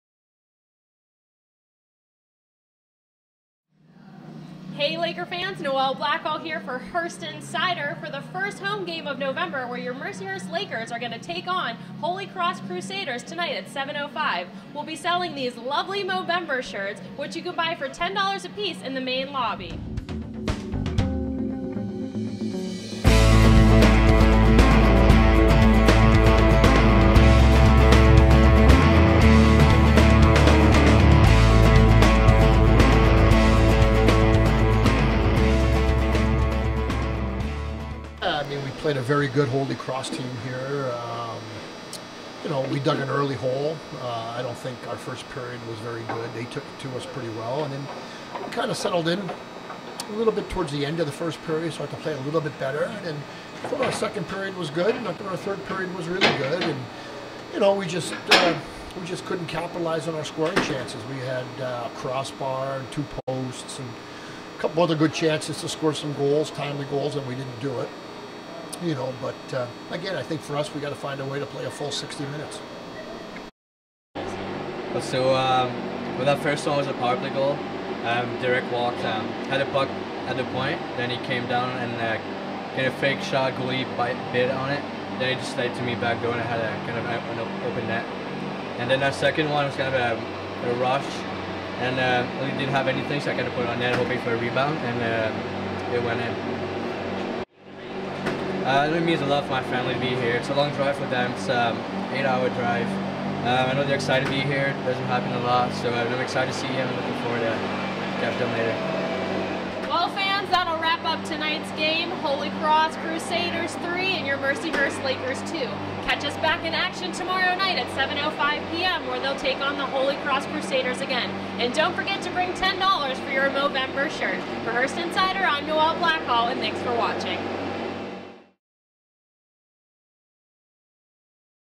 Lakers Drop Game to Holy Cross
The Mercyhurst men's ice hockey team fell to Holy Cross on Friday November 11, 3-2. Interviews with Head Coach Rick Gotkin and Junior Jonathan Charbonneau. Charbonneau scored both of the Lakers goals.